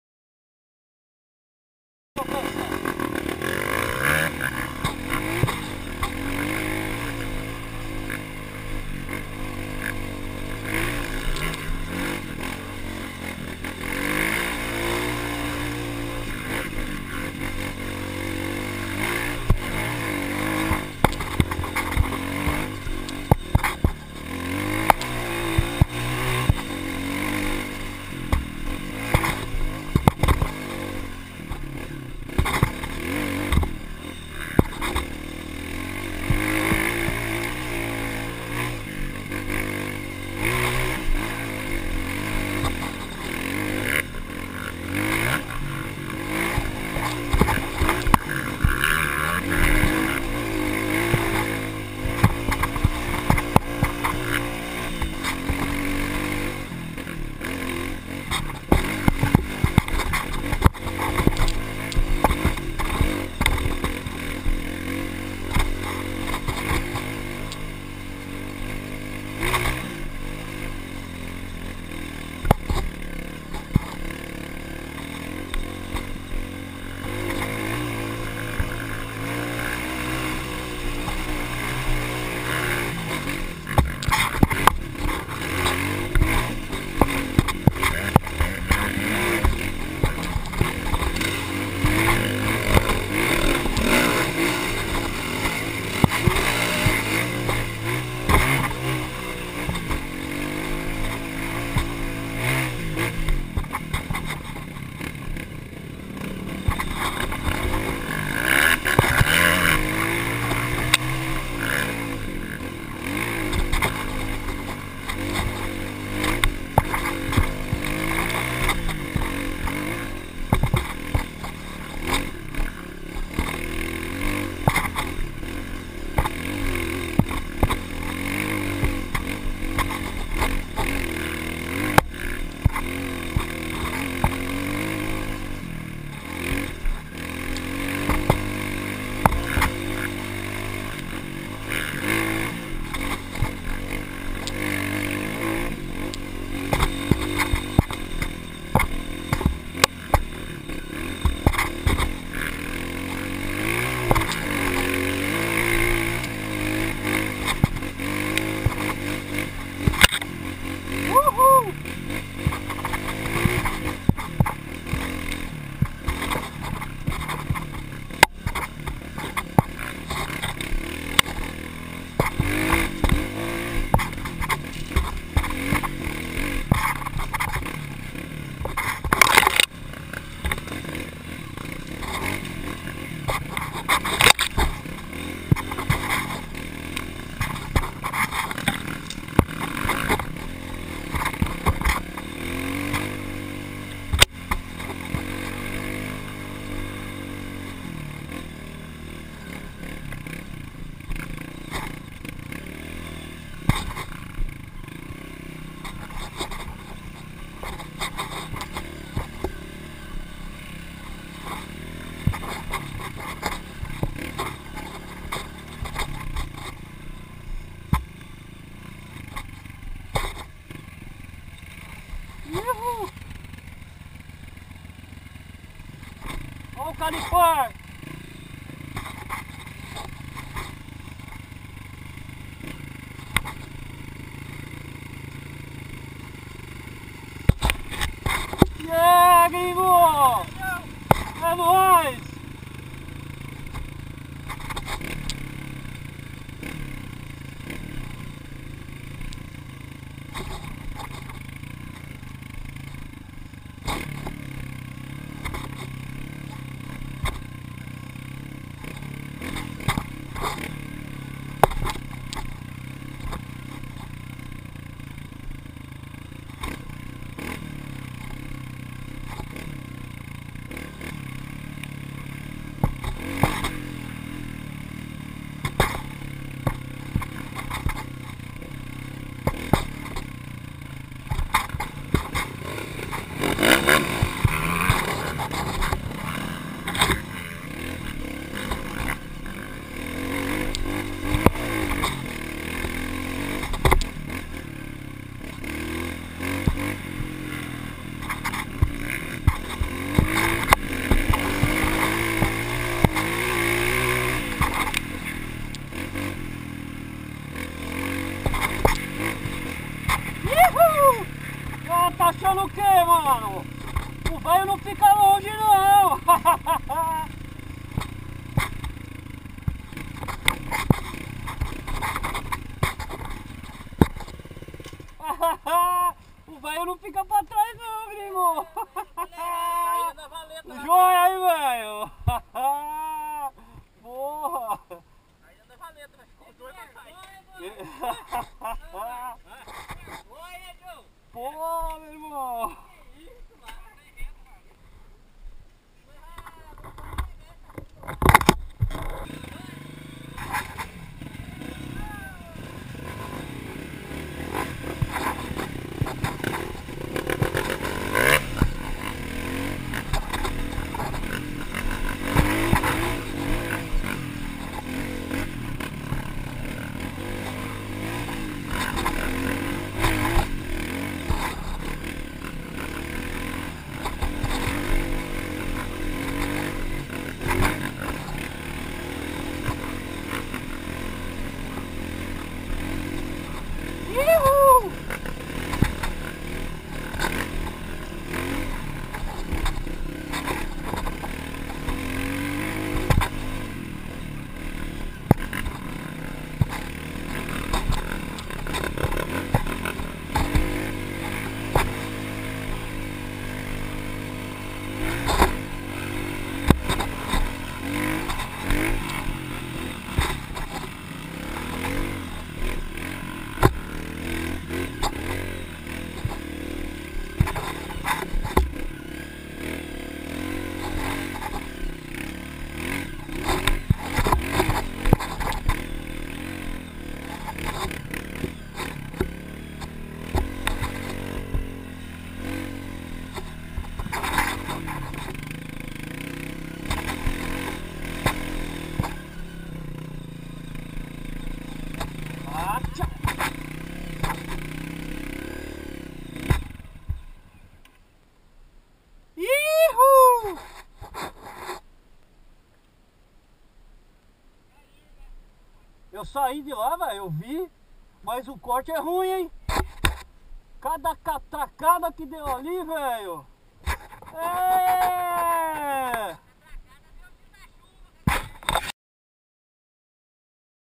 ECOTUCO Trilhas em Borda da Mata MG Serra dos Almas 2 "O Carreador"
Ainda na Serra das Almas, entrando nas terras de nosso colaborador "Robinho", que possui um  desafiador e interminável Carreador. Se liga na tocada do Gringo! Sem falar que foi a 1ªvez que entrei nesse carreador sem fim, além do sol na cara pra detonar!!! Sensacional Pessoal!